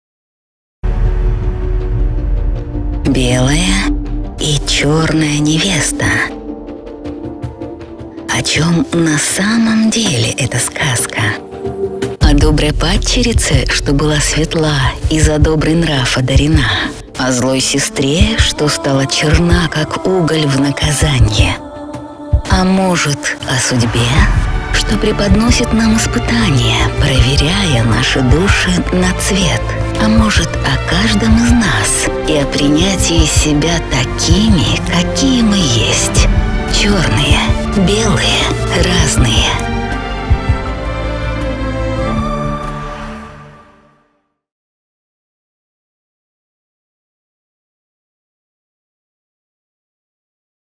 Black and white bride - Valeria Sintsova and Angelina Sakhatskaya
Choreography & dancer: Valeria Sintsova and Angelina Sakhatskaya

There is no person in the world who would not know at least one fairy tale. We all read, listened, or retold the tales ourselves. So we are readers, and storytellers, and storytellers. Every person has favorite fairy tales. A fairy tale has always existed. Listening to fairy tales or telling them, people become aware of themselves, their thoughts, dreams and hopes, their understanding of the Wonderful. But what do we know about our favorite fairy tales from childhood? Was everything really like that? The plot, the actions of the heroes? But this is just quite a layer far from the essence! So it turns out that every time the secret meaning of fairy tales, their true essence eluded us, because not everything is as simple as it seems at first glance.
We will open the magic door to the Magic Land, and she found out that all of this was actually and is being experienced by our heroes ... The heroes of the fairy-tale dance performance "Once upon a time ... or do you know everything about us fairy tales? "
Annual Tribal Festival Full Moon 2016.
Welcome to the Tale!